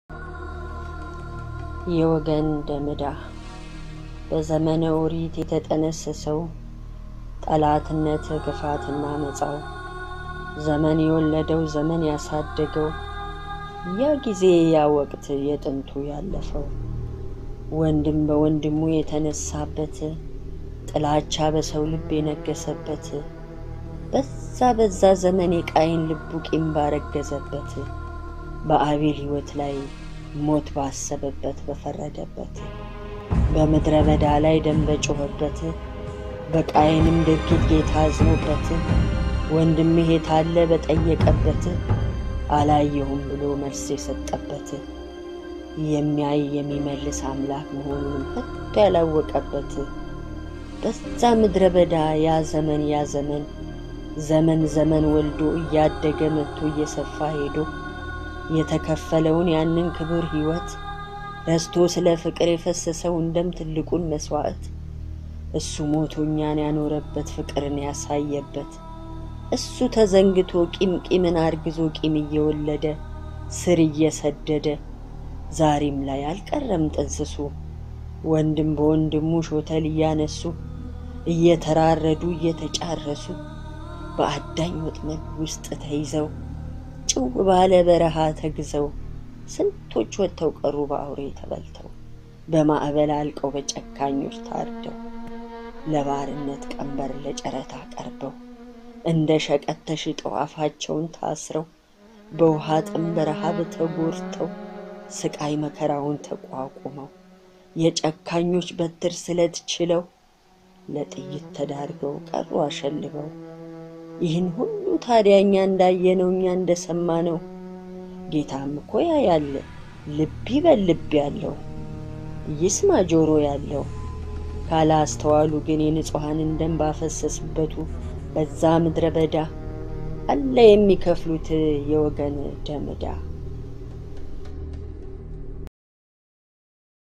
Amharic Poem - Yewegen Dem Eda (የወገን ደም ዕዳ) by Lina Endale
A dedication to those who were/are being sold as slaves in this time and age